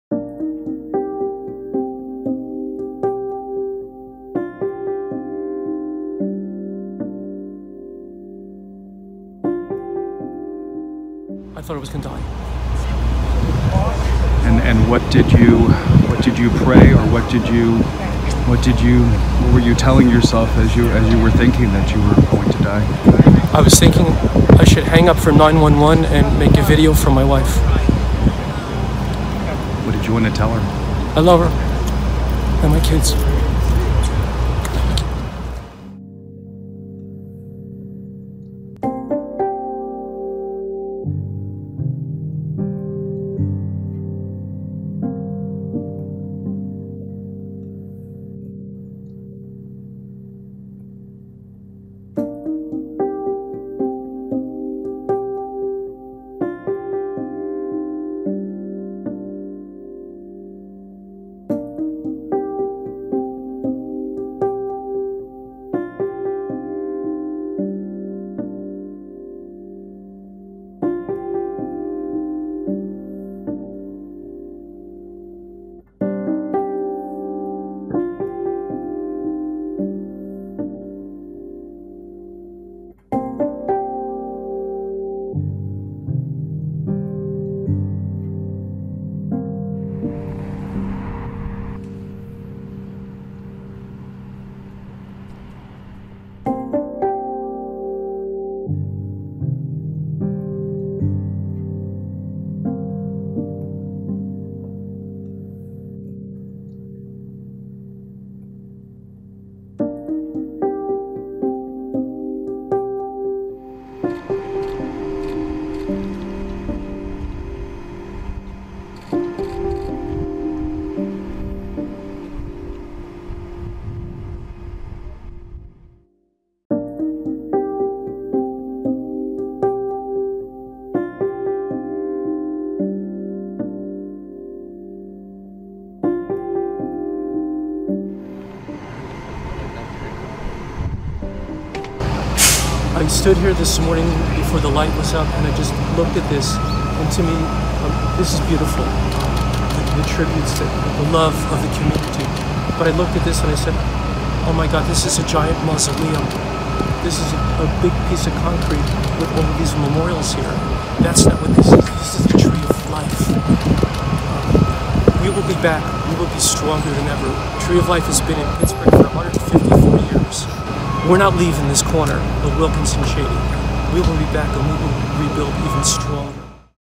Rabbi relives horrors of Tree of Life shooting
(30 Oct 2018) Only days before a gunman entered the Tree of Life Synagogue, Rabbi Jeffrey Myers had posted an online blog encouraging his congregation to make the most of life's moments. On Saturday, he found himself barricaded in a choir loft bathroom. (Oct. 30)